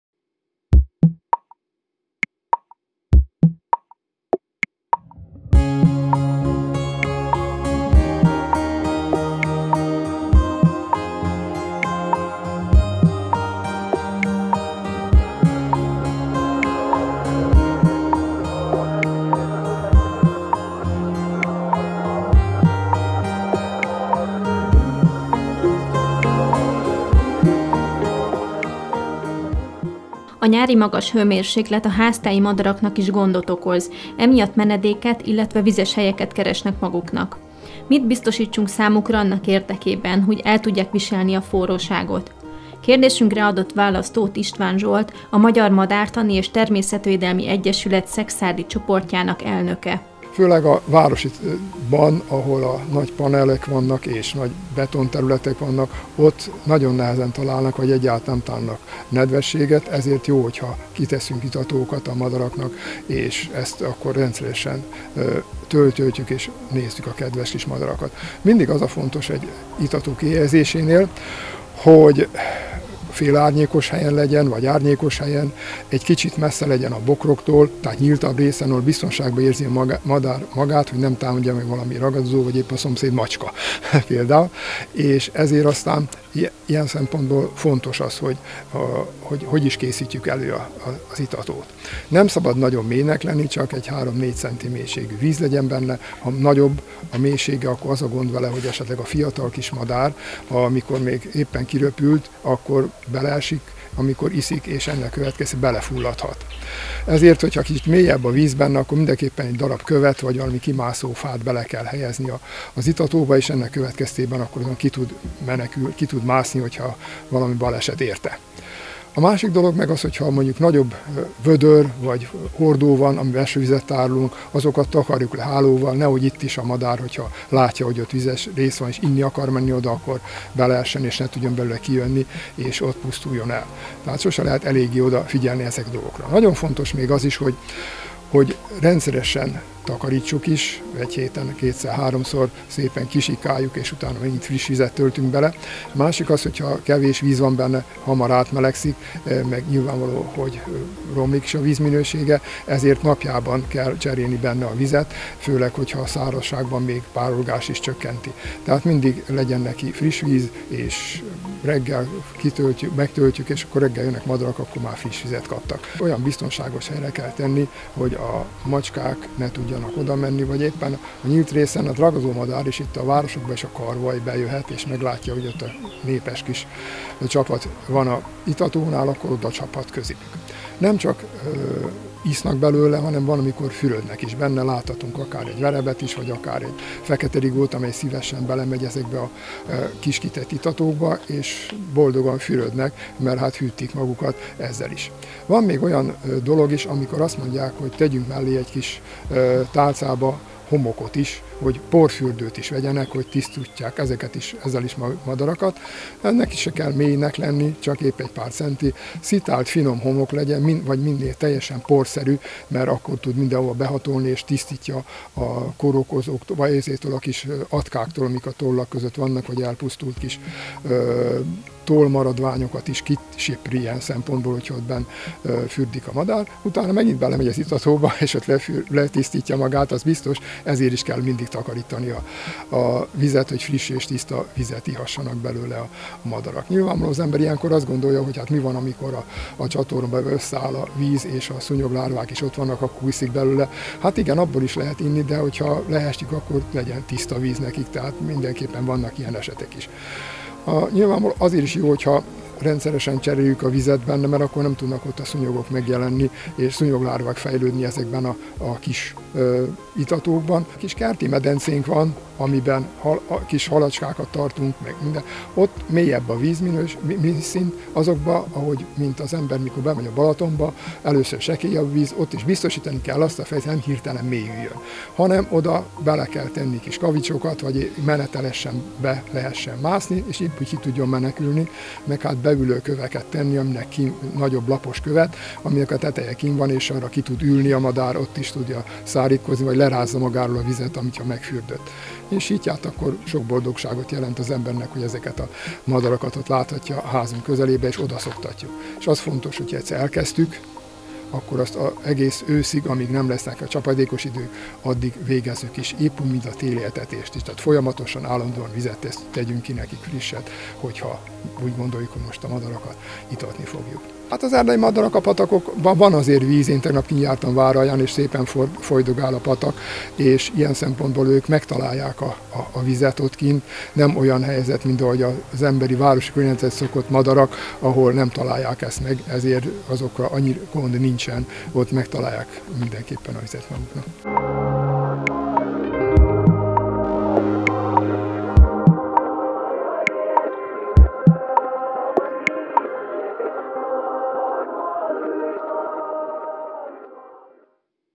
Nyári hőség - hogyan segítsük a madarakat?
Az idei nyáron a hőmérséklet gyakran 35 fok fölé is emelkedett. A hőség nem csak az emberi szervezetre gyakorol nyomást, hanem a természet élőlényeire is, így a madarakat is megviseli a szélsőséges időjárás.